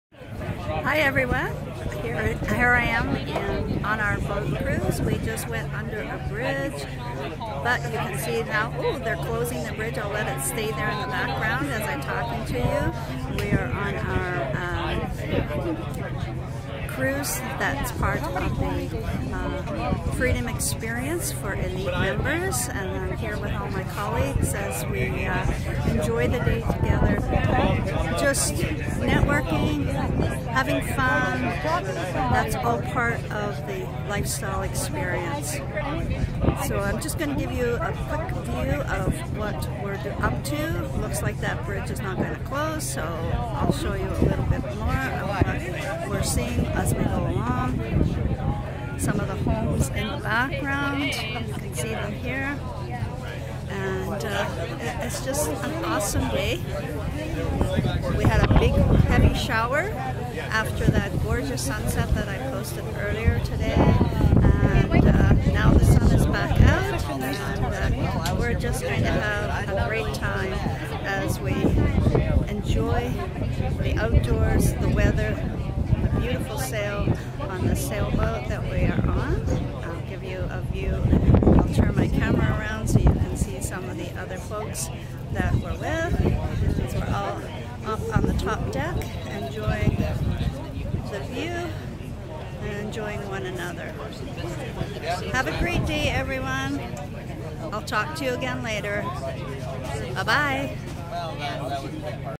#03 - Month Long Trip - June 2017
Enjoying an awesome day with the Elite Membership of LDI. This was our time to play after three days of training at our Freedom Experience. Check out my site and find out how you can enjoy living the life of your dreams.